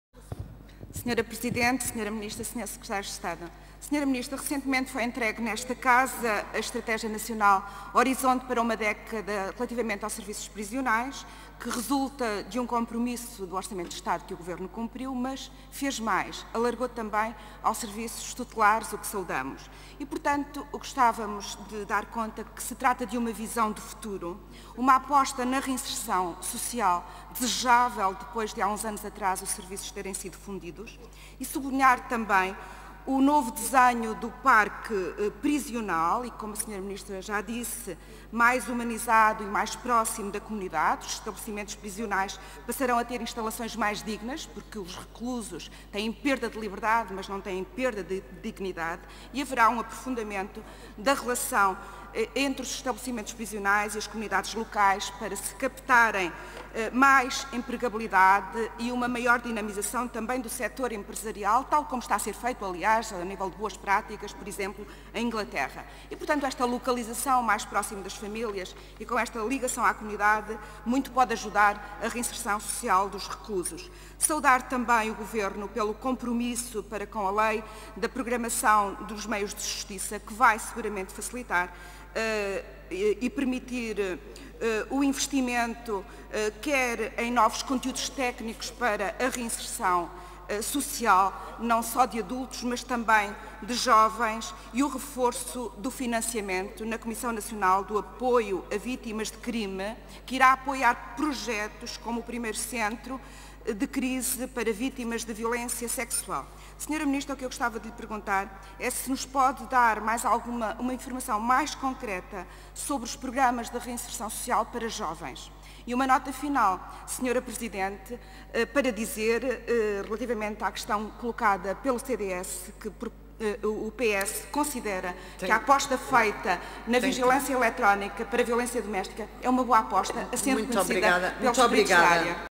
10-11-2017 | Debate na Especialidade OE2018 - Audição Ministra da Justiça | Elza Pais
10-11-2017 | Intervenção da Deputada Elza Pais - Debate na Especialidade do Orçamento do Estado para 2018 - Audição da Ministra da Justiça Francisca Van Dunem - Comissão de Orçamento, Finanças e Modernização Administrativa e Comissão de Assuntos Constitucionais, Direitos, Liberdades e Garantias